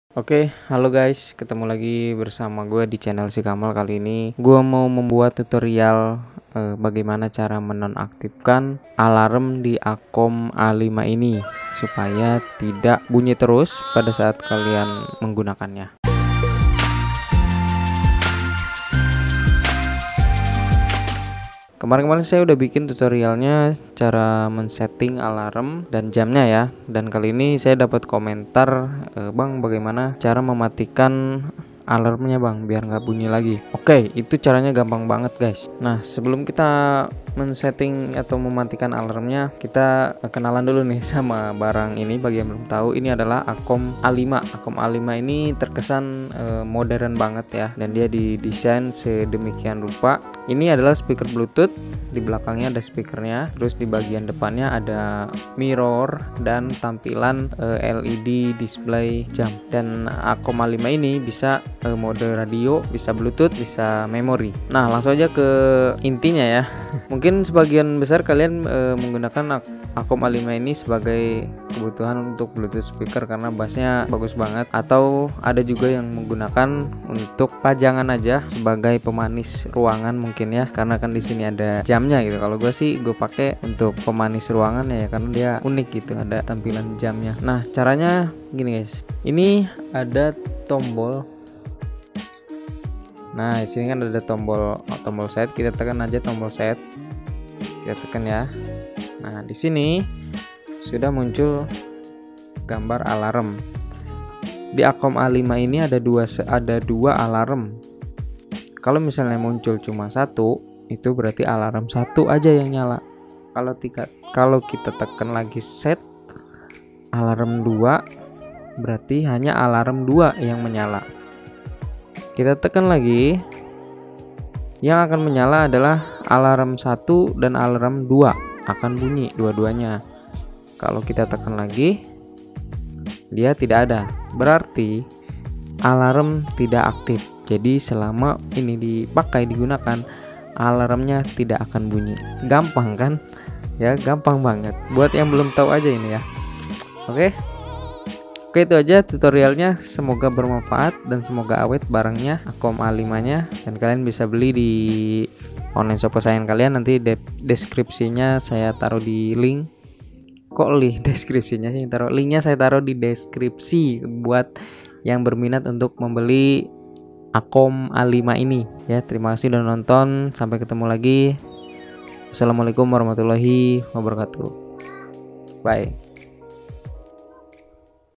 Matiin Alarm Cara Setting Alarm Acome A5
Begini cara matiin alrm acome A5, sangat mudah cara menonaktifinnya